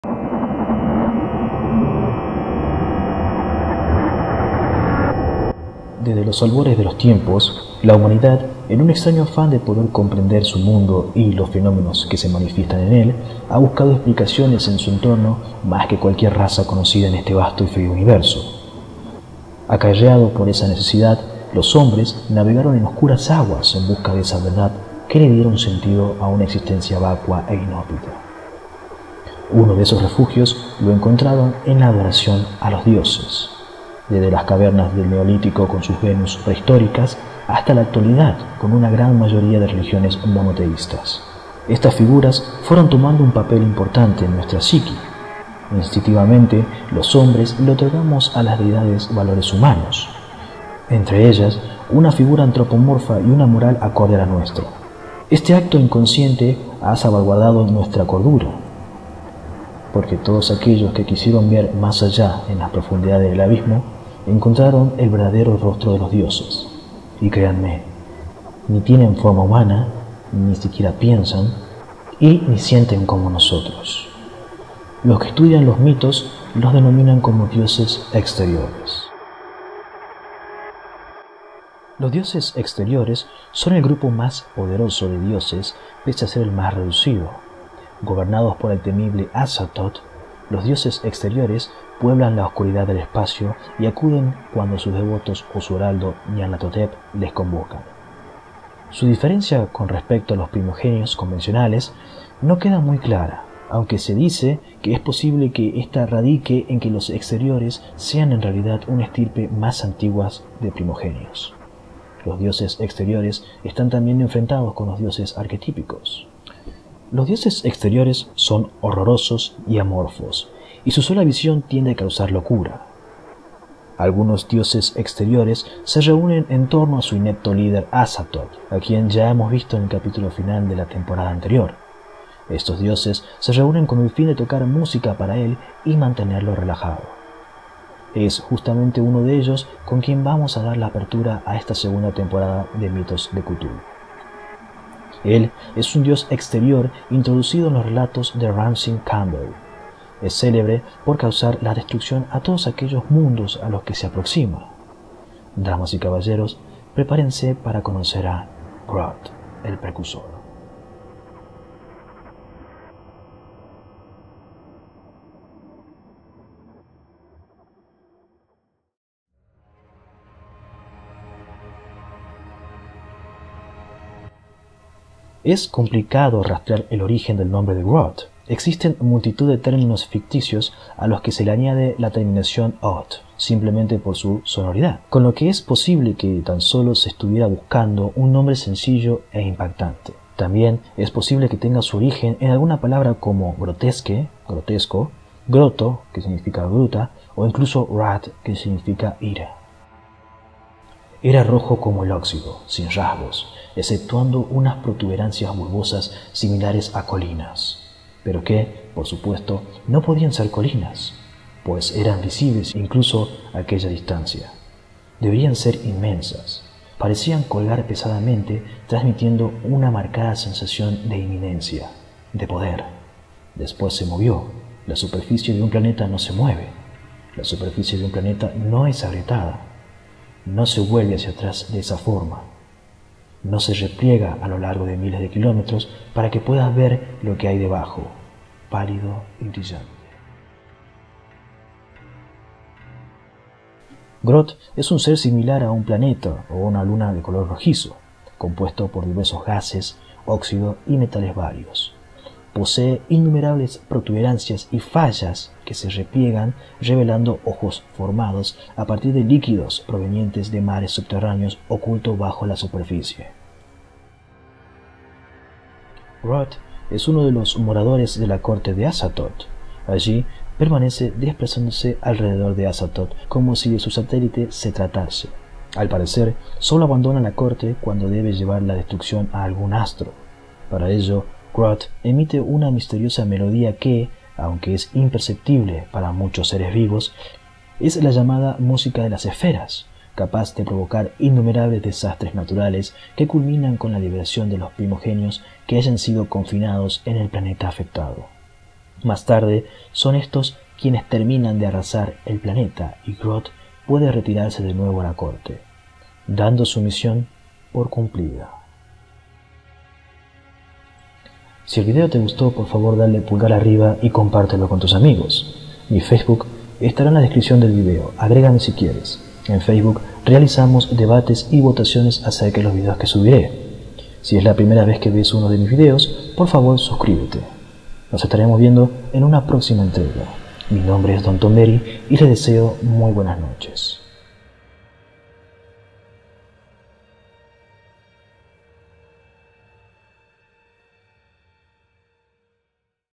Mitos de Cthulhu Capitulo 12: "Ghroth, El precursor"
Él, es un Dios Exterior introducido en los relatos de Ramsey Campbell. Es célebre por causar la destrucción de todos aquellos mundos a los que se aproxima. Damas y caballeros preparensen a conocer a… Ghroth el precursor.